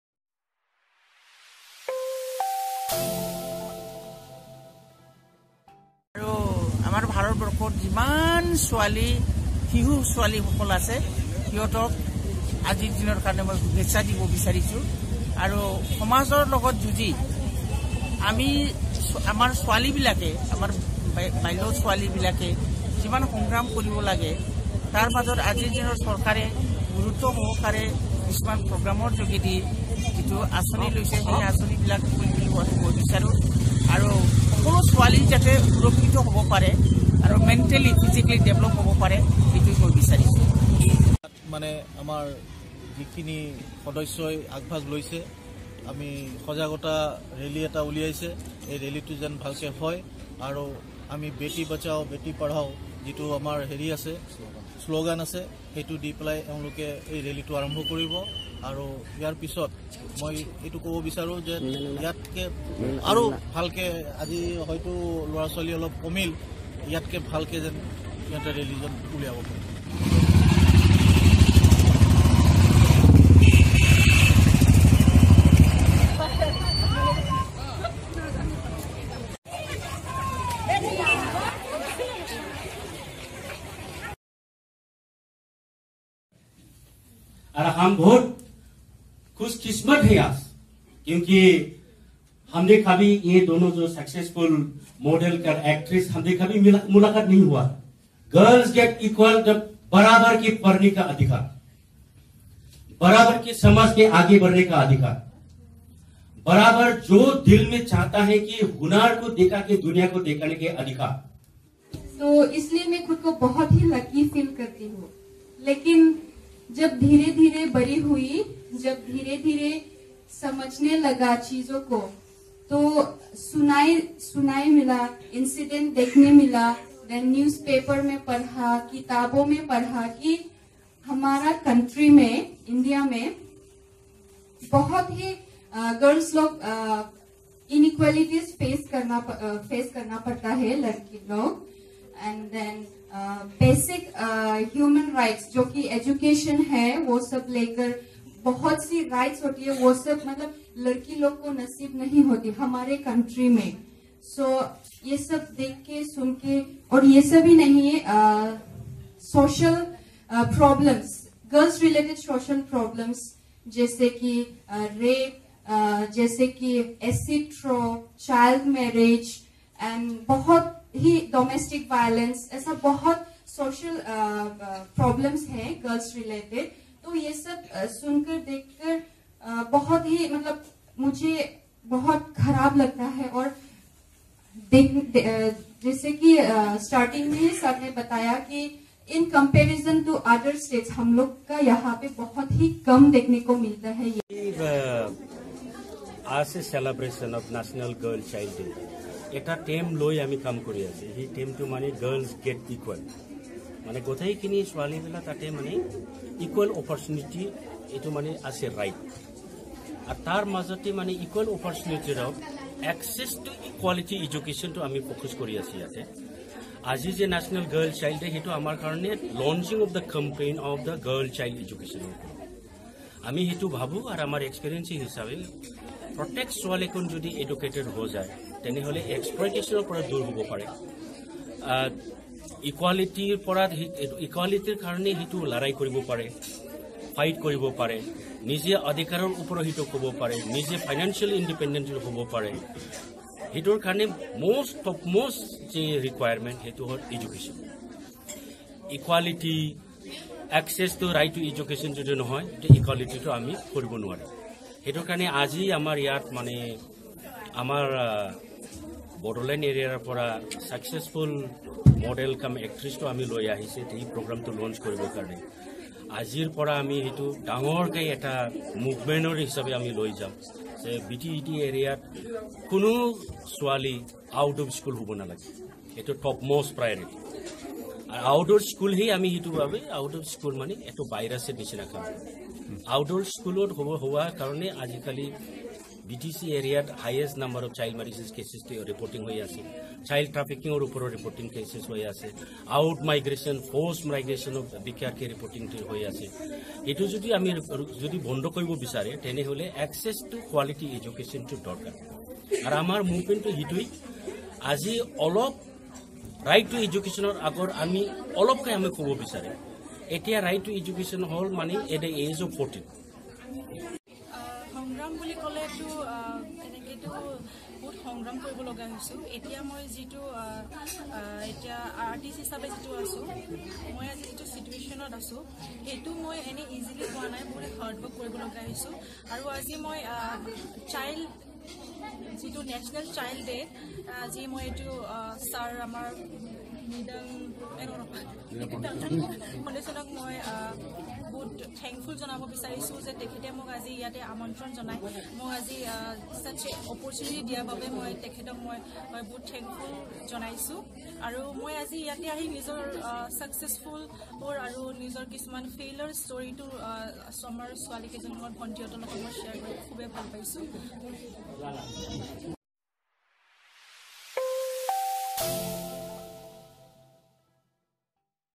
National Girl Child Day// Nedan// Kokrajhar girl's college// Kokrajhar
The National Girl Child day was celebrated in various part of Kokrajhar with a day long programme.

An awareness rally was carried out in Kokrajhar town where over a hundred students participated.  The Bodoland Riders Motorcycle Club and Eastern Riders also took out the bike rally with the objectives to spread awareness on girl child.

The rally was flag off by the Pranjal Sarma, Asst, Deputy Commissioner (AADC) Kokrajhar district. Chairperson of Child welfare committee Kokrajhar Malati Rani Narzary.

Malati Rani Narzary, Child Welfare Committee Kokrahar member Babli Basumatary District Social Welfare officer Gunindra Talukdar were among those present.

Malati Rani Narzary stressed the importance of giving equal importance to girl child saying that the girl child must be protected and crimes on girls should be stopped. She has emphasized healthy integration of the society and girl child must be uplifted through the education, social development.

The Nedan Foundation, a Kokrajhar based NGO, also celebrated the day at Hotel Gaurang Valley among the adolescents girl from different parts of the district where over 60 girl child participated. Well-known Bodo film actresses and Youth icons- Helina Daimari, Lipika Brahma and Pansy Brahma attended the programme and interacted with the children.